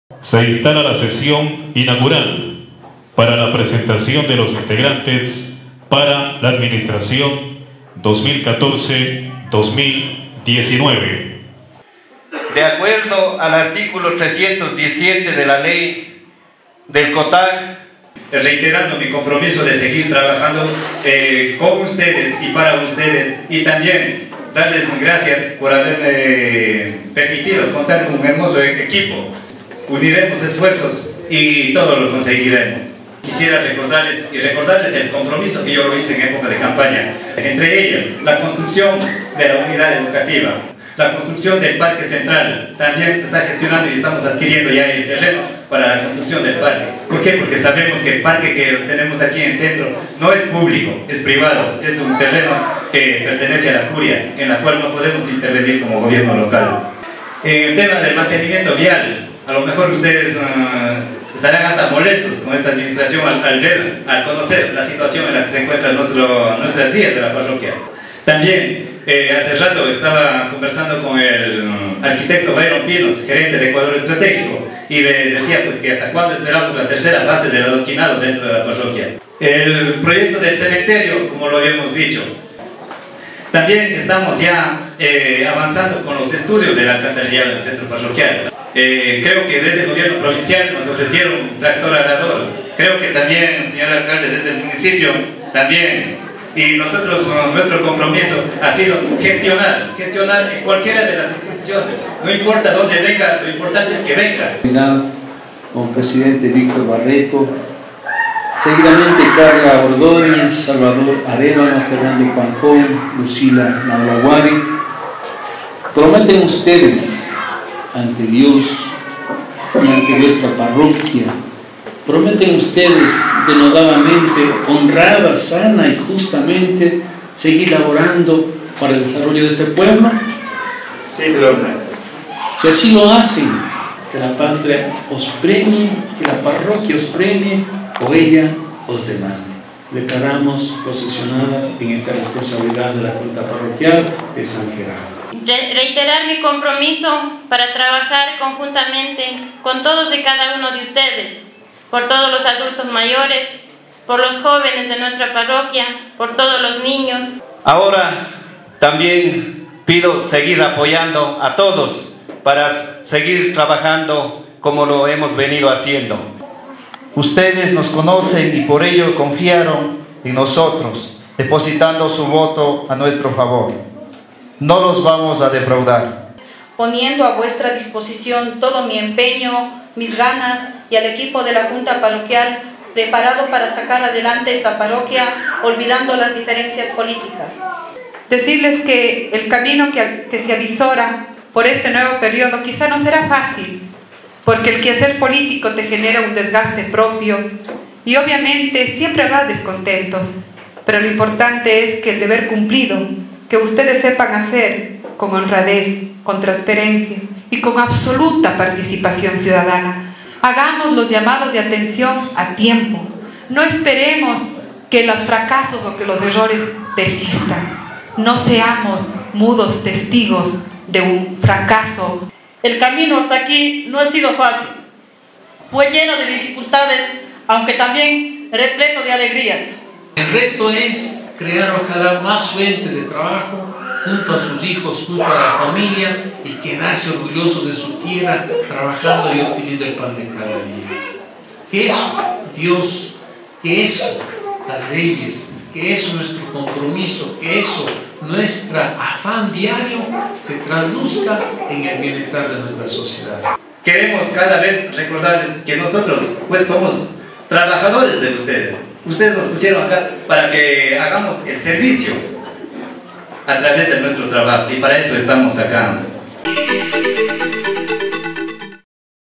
Posesión de autoridades seccionales en San Gerardo
El miércoles 14 de mayo, en el edificio de la Junta Parroquial de San Gerardo se posesionaron los 5 representantes de la nueva junta parroquial, 3 de los cuales fueron reelectos, la asambleísta Liliana Guzmán y autoridades provinciales afines al gobierno participaron en la ceremonia. Web